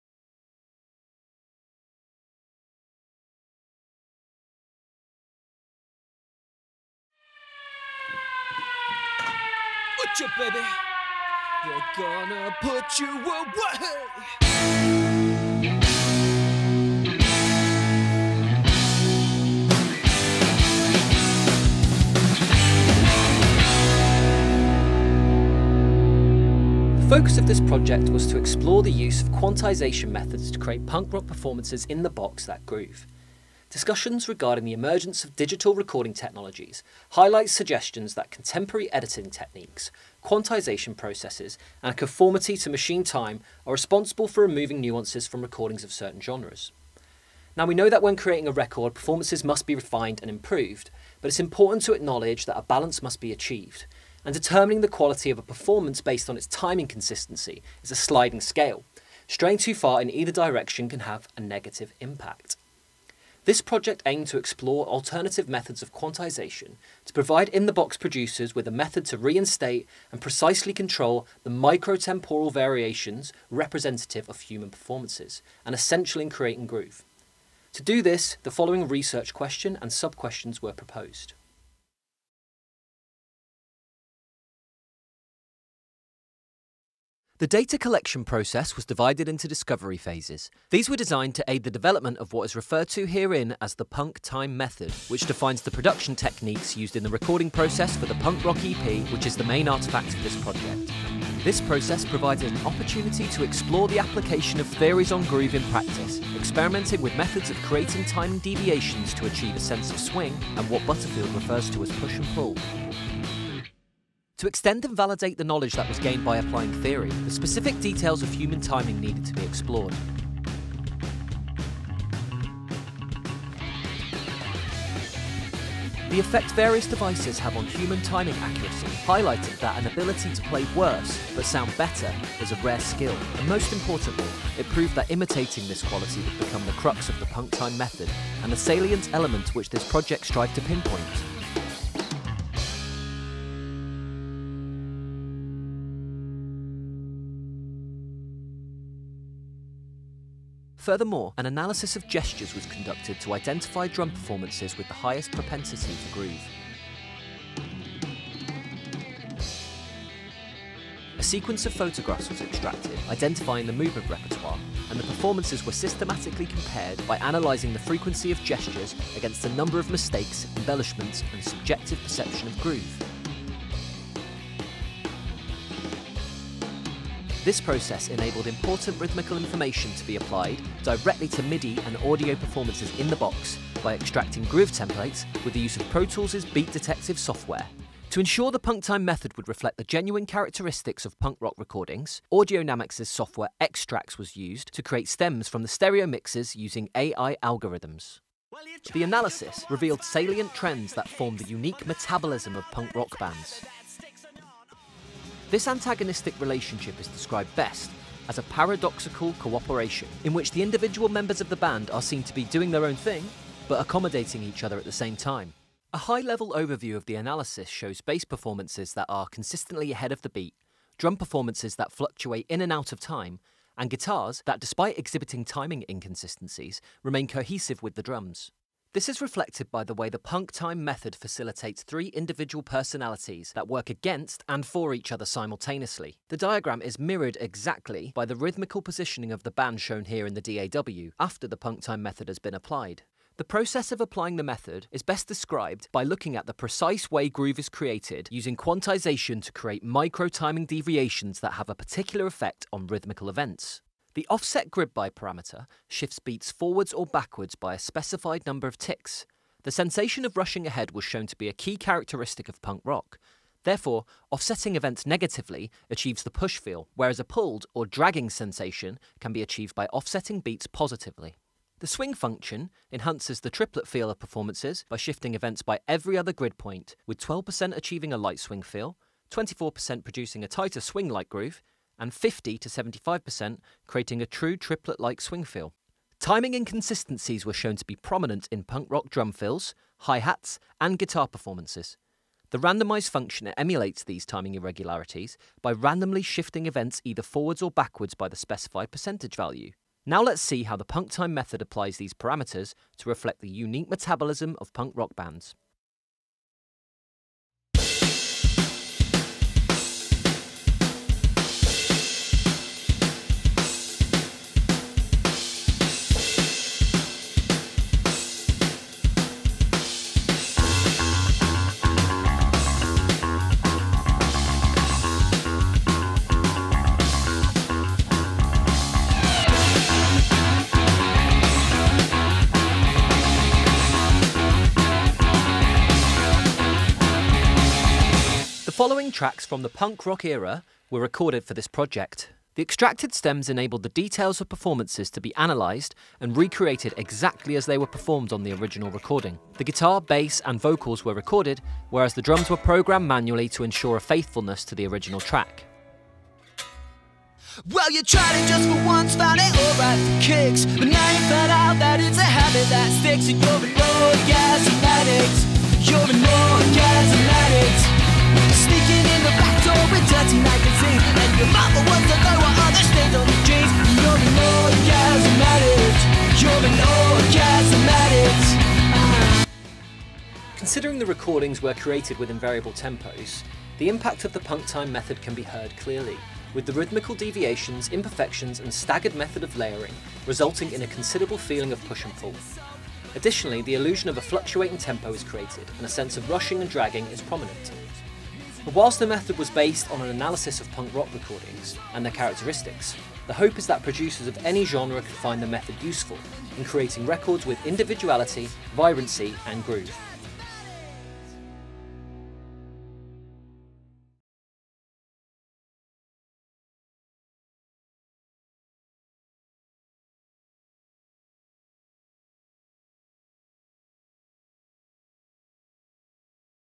Jonathan Gaskin (MA Record Production)
Jonathan Gaskin (MA Record Production) - Creating Performances ITB That Groove and Applying the ‘Punk Time’ Method

The purpose of this study is to explore further the use of quantisation methods to create performances ITB that groove. An analysis of recordings from the punk rock era provides insight into the salient characteristics of punk rock performances at an atomic level, forming the basis for the ‘Punk Time’ method. A comparison of devices enables the nuances of performances to be examined, and a study into the impact gestures have on performance
quality establishes a foundation for ensuring digital performances groove in a human way. Furthermore, the project evaluates the application of the ‘Punk Time’ method by recreating three recordings from the punk rock era.